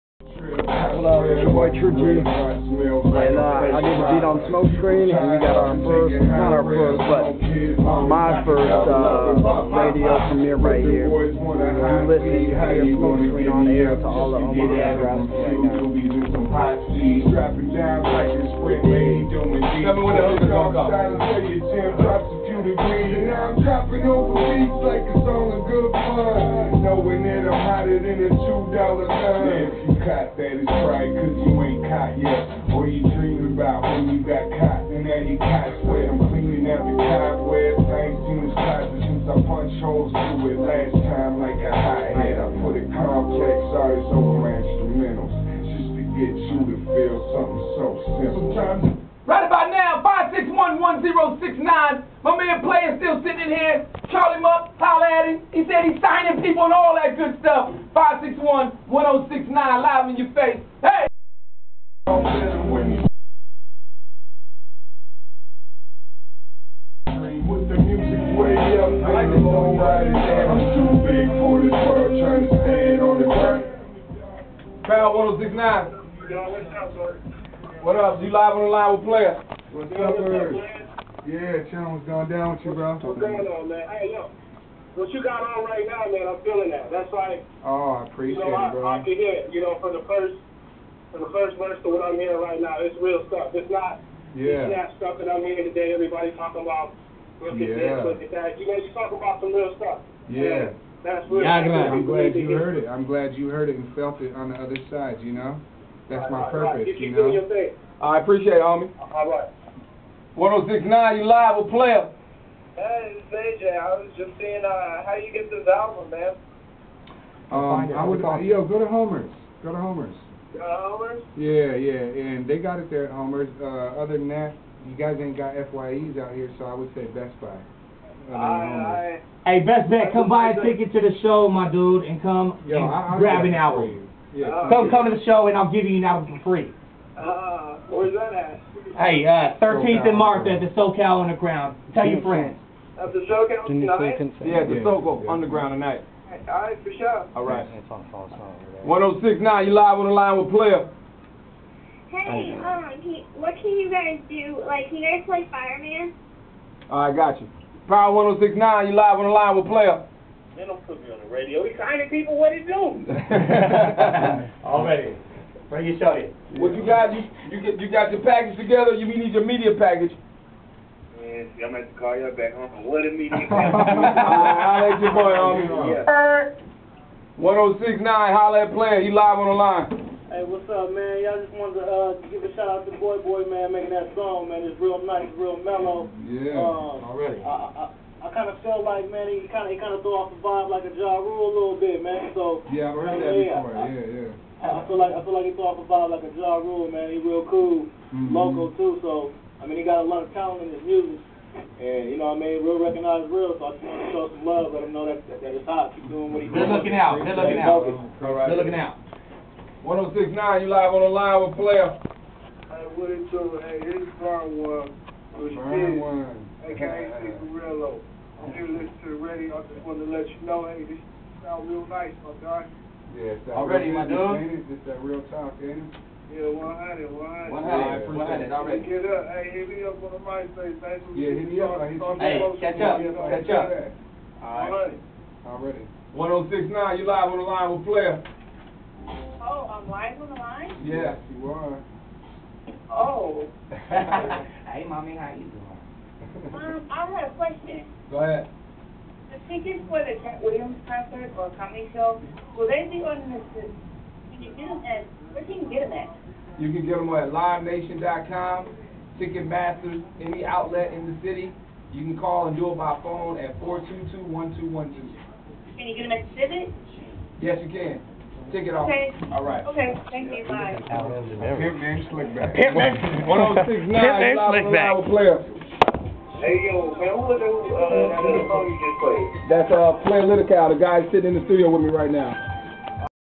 Power 106.7fm Interviews Playalitical live in the on air studio.
DJ Sol of Power 106 interviews Playalitical. Callers call in and Playalitical takes thier questions.